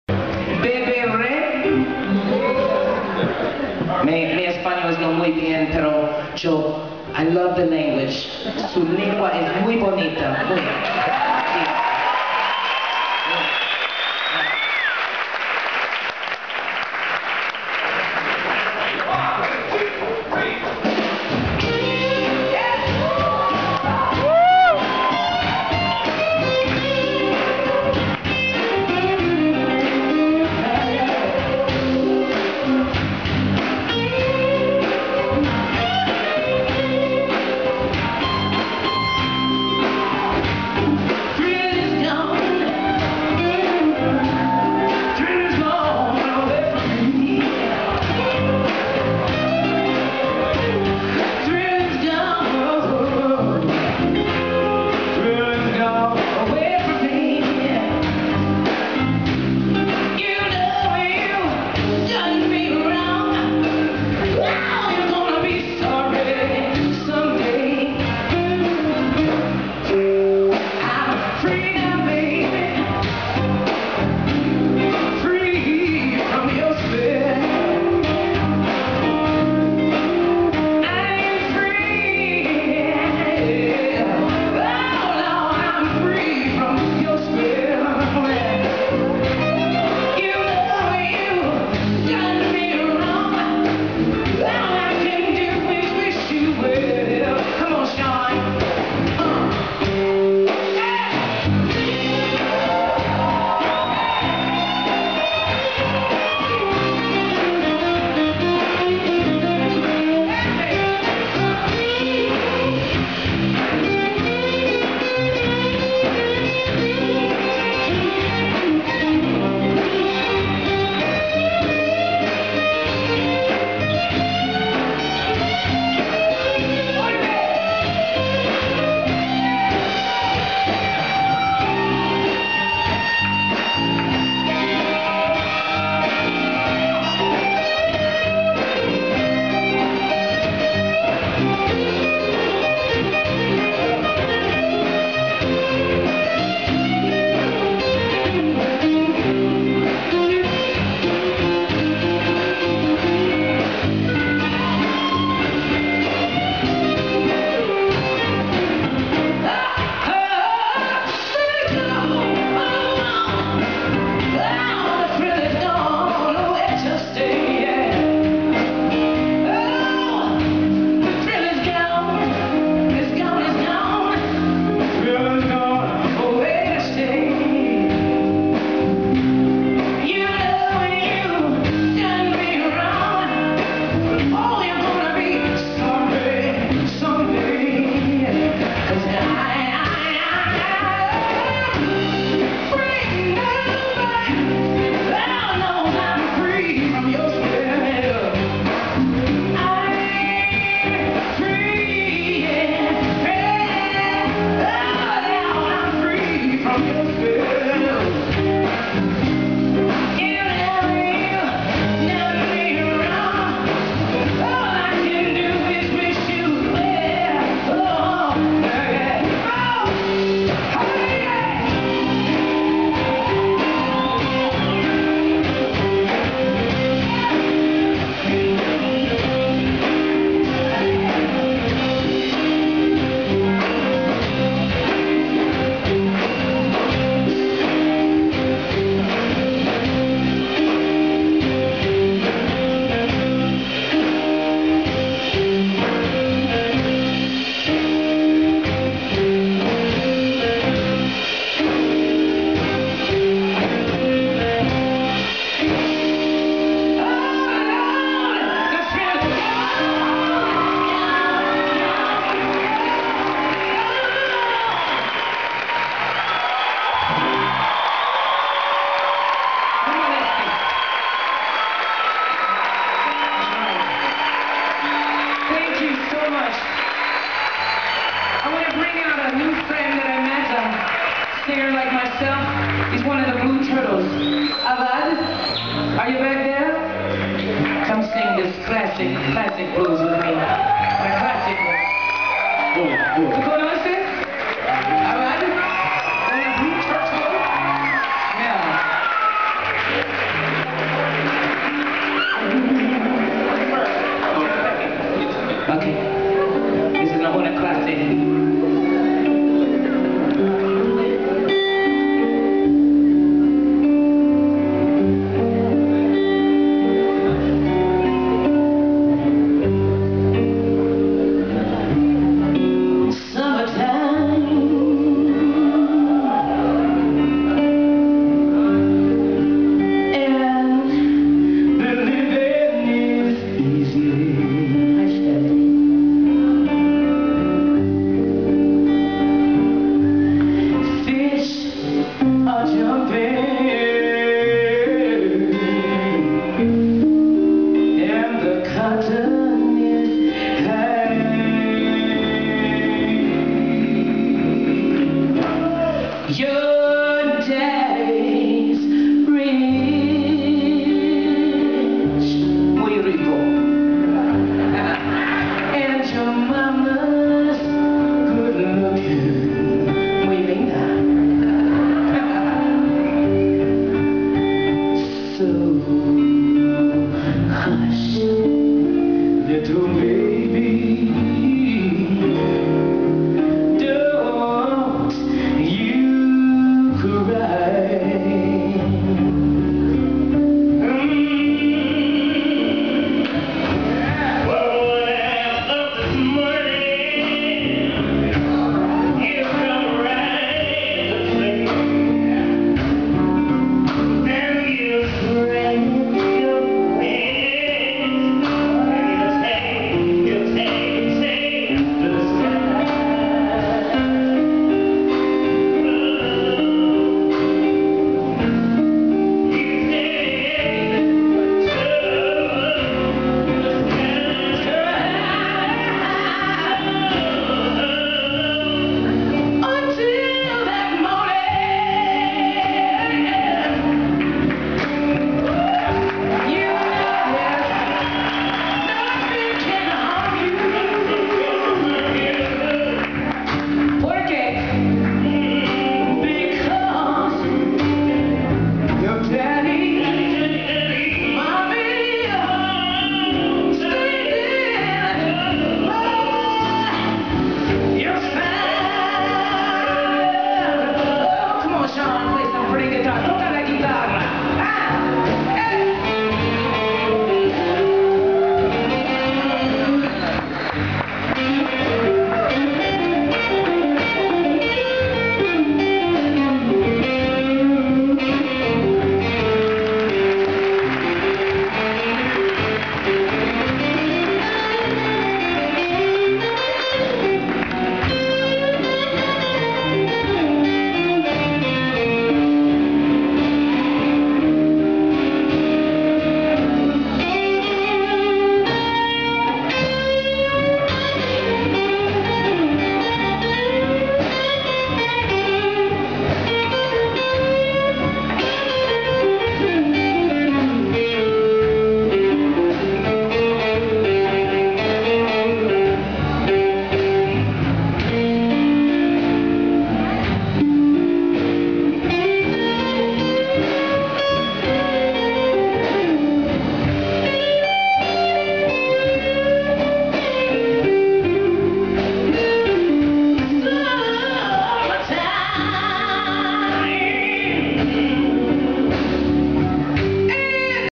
Shaun Booker + Little Joe McLerran band | Cali Blues Festival!
Shaun Booker, Sean Carney, Little Joe McLerran Band en el cierre del Cali Blues Festival 2012, organizado por el Centro Cultural Colombo Americano de Cali.